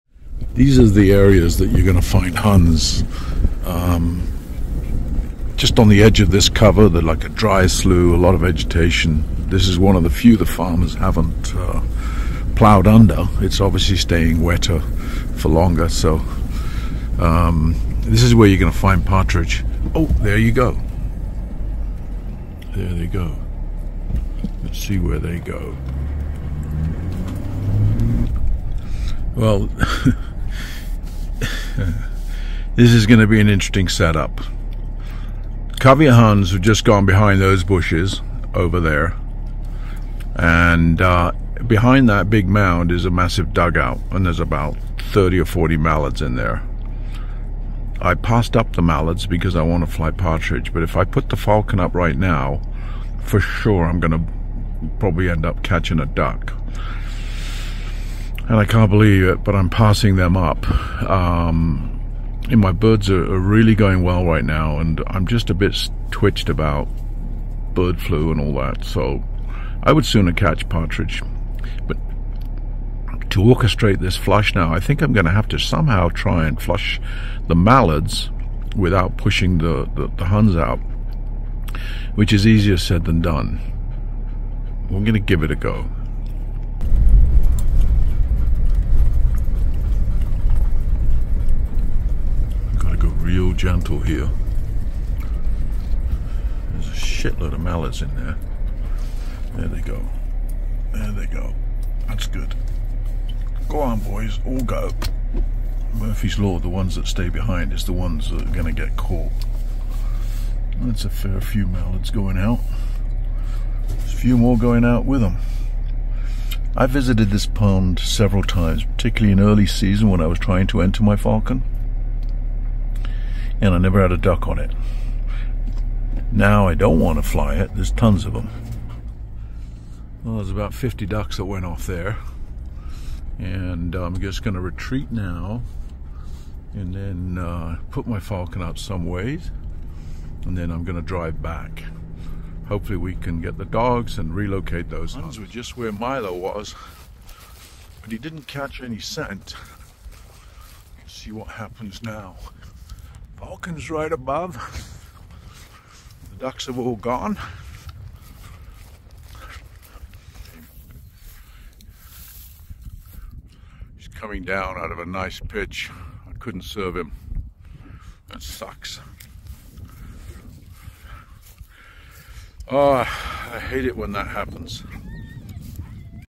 Win Some Lose Some .... Orchestrating a hun flight in a duck set up HD 720p
I took a gamble on this set up. Having just passed up the dug out full of Mallards I press on in search of partridge. Sure enough I bump a covey of huns nearby and they go right next to the dug out full of ducks.  I'd rather not fly the ducks but because their mallards, they'll flush readily and my plan was to gently flush them and then put up the falcon and fly the huns.  It sounded do-able to myself and it nearly worked but in my attention on the ducks I did'nt notice the huns slip out the back door.

Some you win, some you lose .